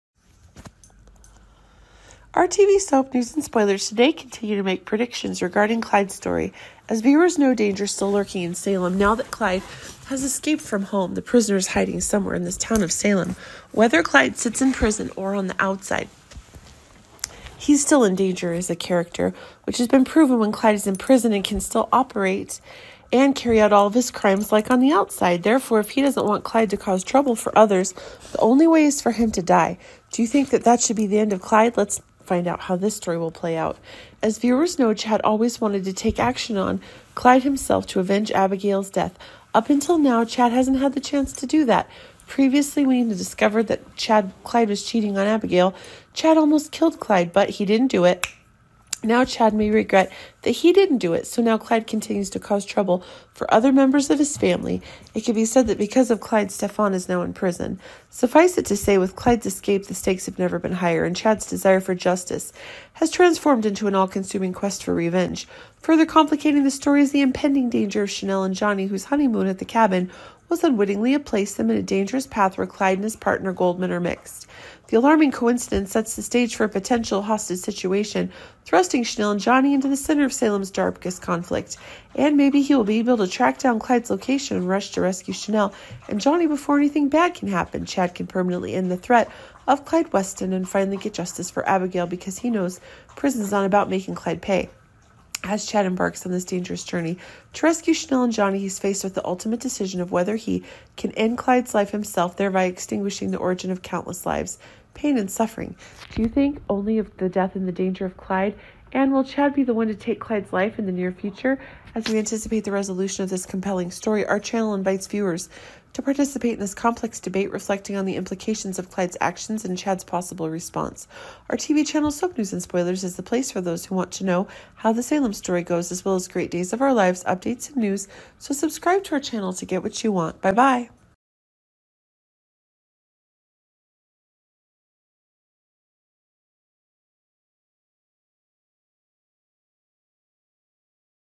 OMG! Days of our lives spoilers: Chad finished the story of Clyde, whose death came unexpectedly
Our TV soap News and Spoilers today want to share with you the prediction of Abigail's return. Do you think we'll see Marci Miller return to her role as Abigail next week? Why do we make such predictions? As we delve into the possibilities and hints scattered throughout recent episodes, let's explore tantalizing clues that suggest a dramatic twist could be coming. Let's find out if this story happened or not.